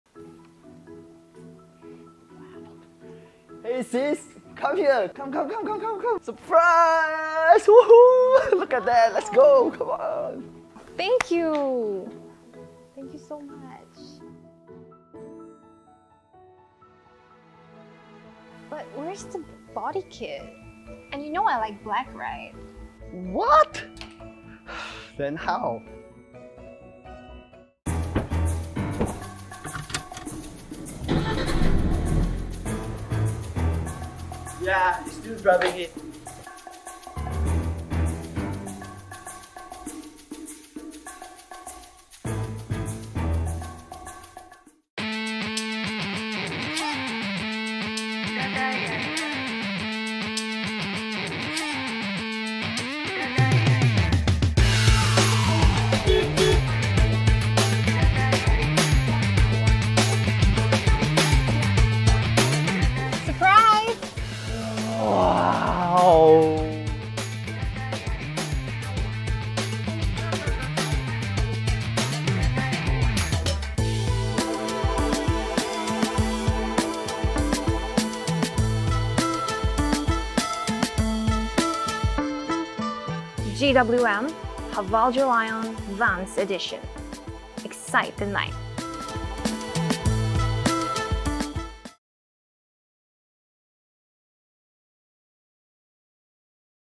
GWM HAVAL JOLION VANCE | Excite the Night
Introducing our all new JOLION VANCE Edition. Ignite the light, excite the night.

Ready to conquer the road with the newest sleek and stylish outlook of GWM HAVAL JOLION VANCE.